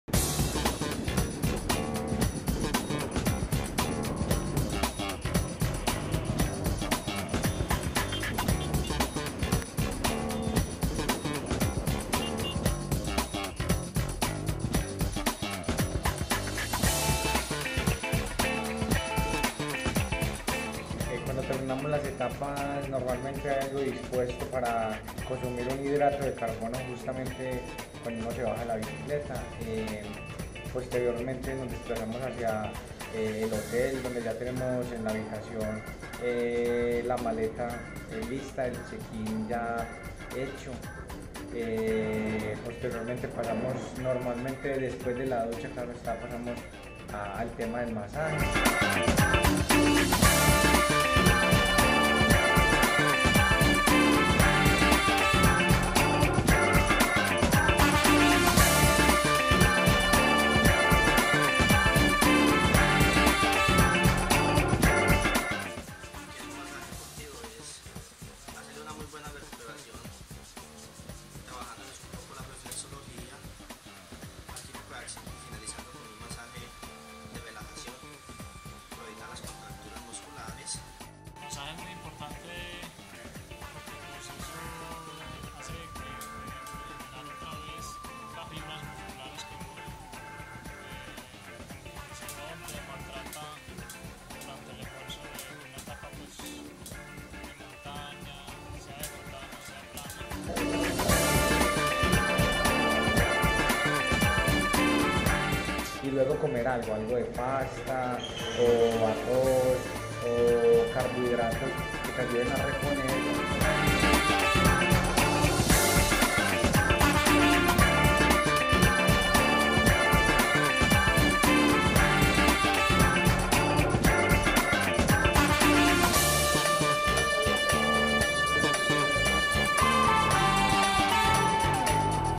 Vuelta a Colombia 2016. Etapa 2.
Luego de un arduo esfuerzo en una etapa con mucho calor y humedad, lo que se hace luego de competencia es lo más importante para poder recuperar  el cuerpo y estar listo para el siguiente día de carrera.